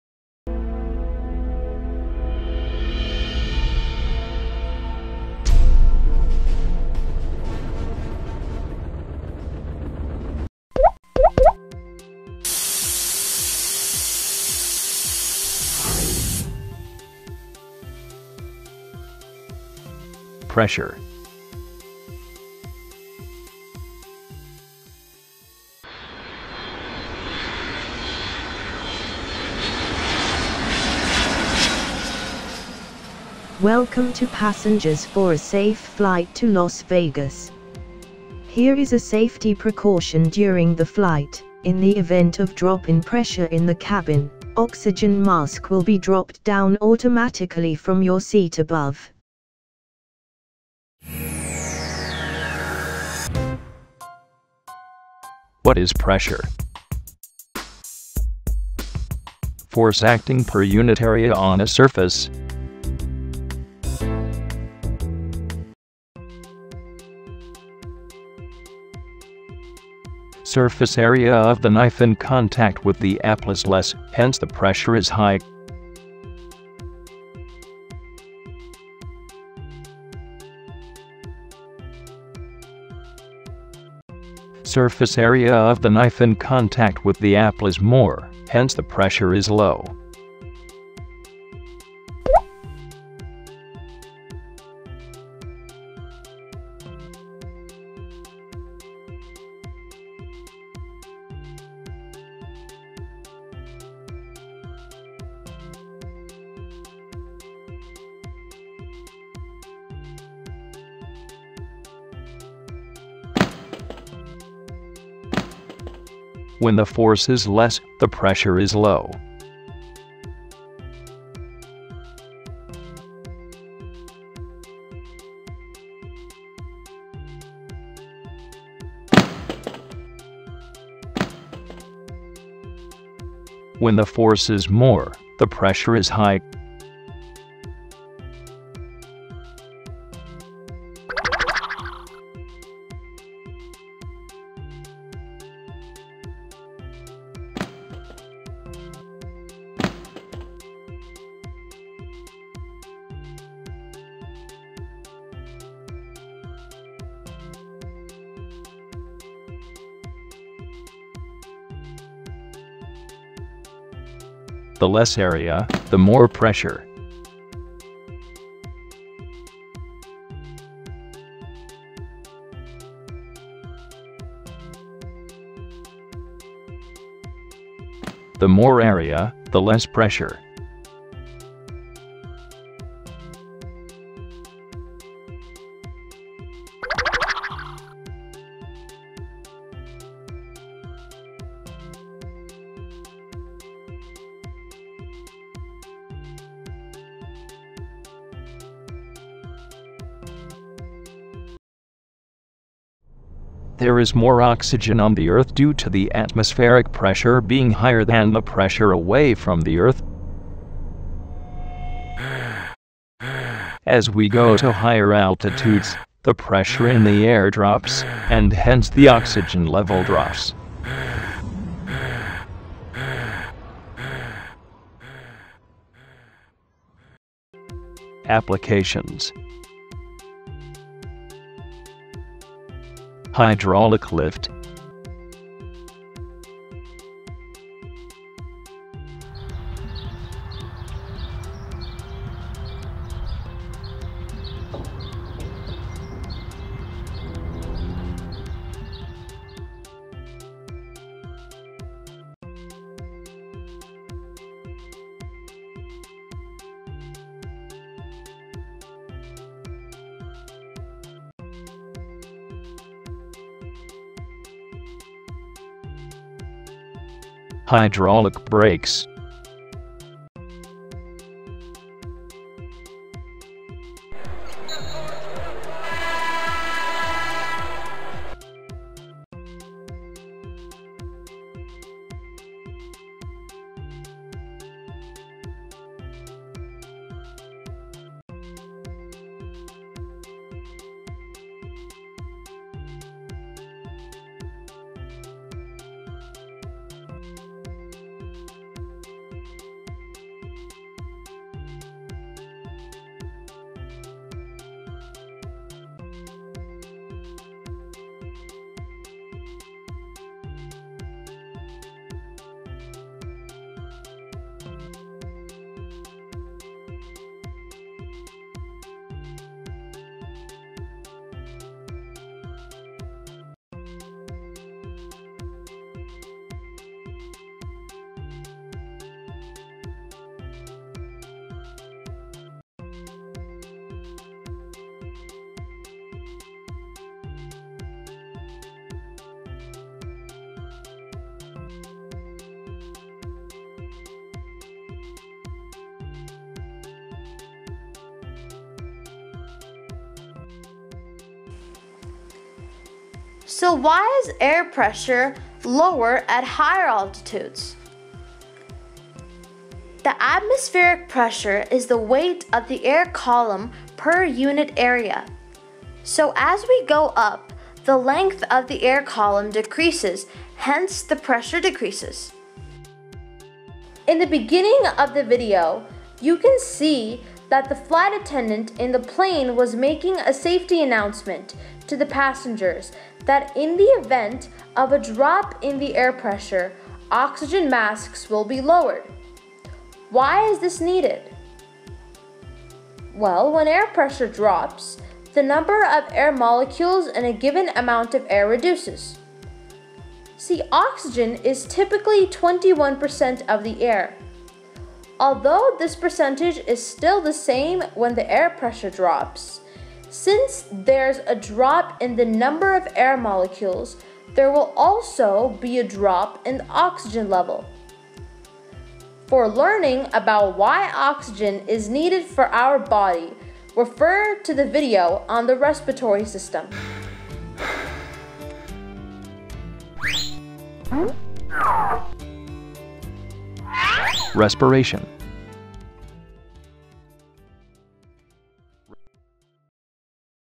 Pressure -- For middle school physics students(grades 6 to 8)
Explain with the help of animation that when some force is applied on an area it exerts pressure.